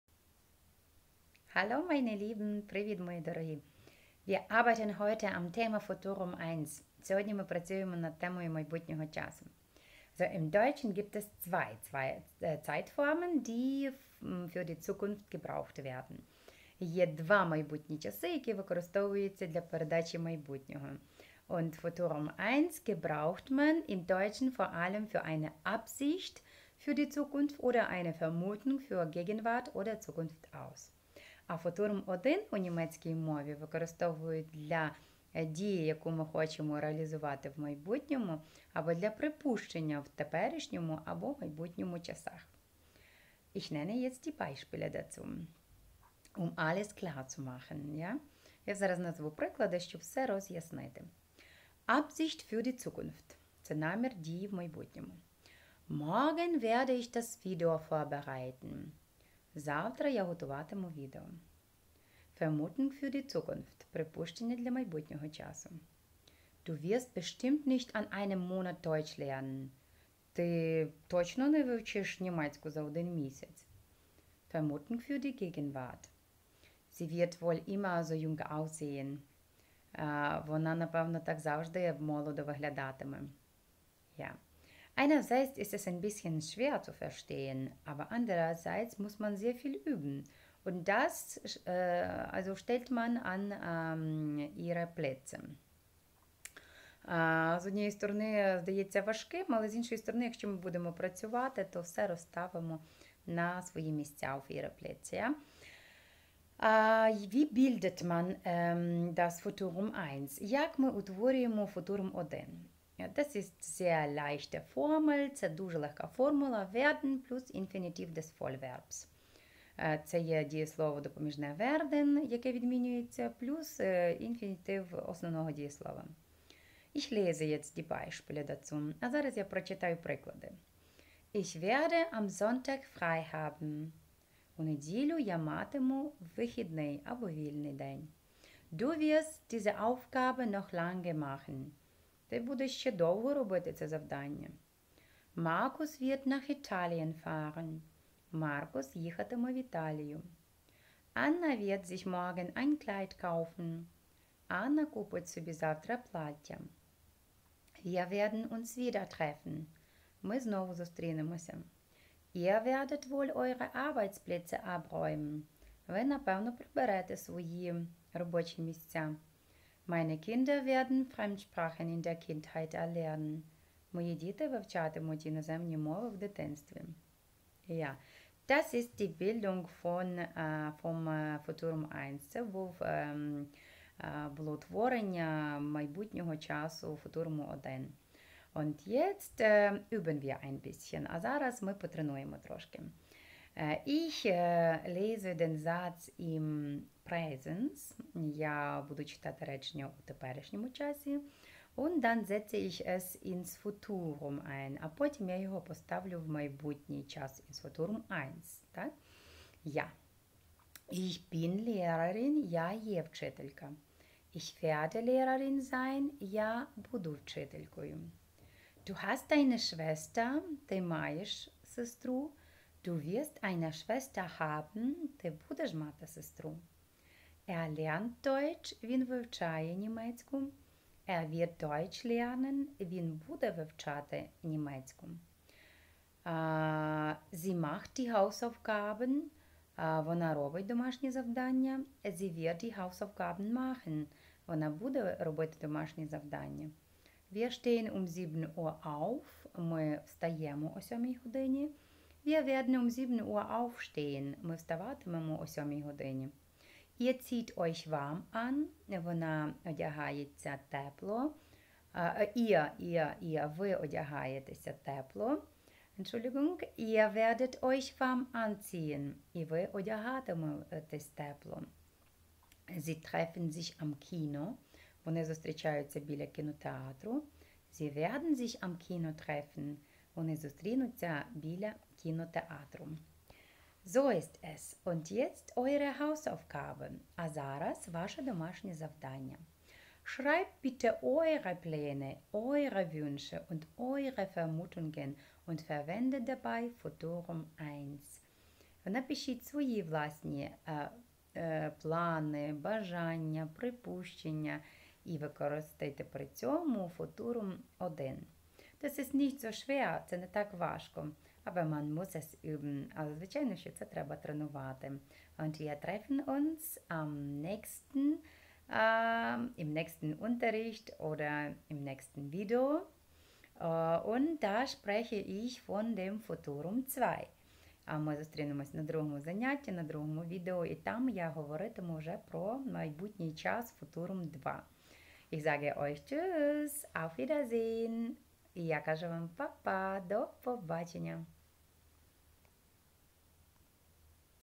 Stunde 31.Урок 31. Futurum I. Майбутній час.
Майбутній час в німецькій мові. Пояснення з прикладами і перекладом.